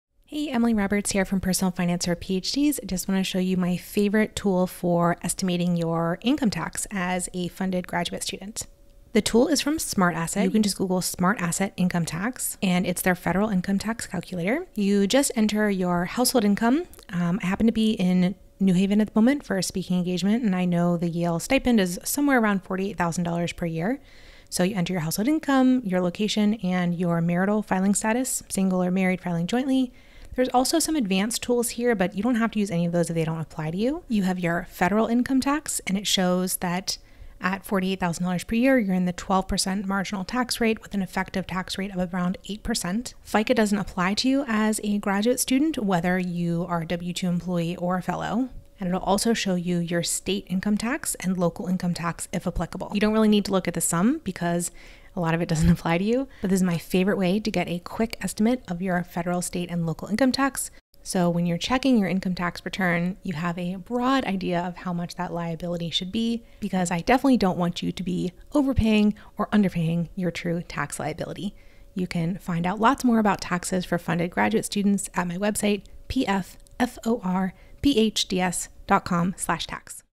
Best Income Tax Calculator for Funded Graduate Students
This calculator is appropriate for funded graduate students in the US who are US citizens, permanent residents, or residents for tax purposes.

Use this calculator to estimate your income tax liability. FICA doesn't apply to a full-time graduate student's stipend, whether they are an employee or fellowship recipient.

More info on taxes for funded graduate students in the US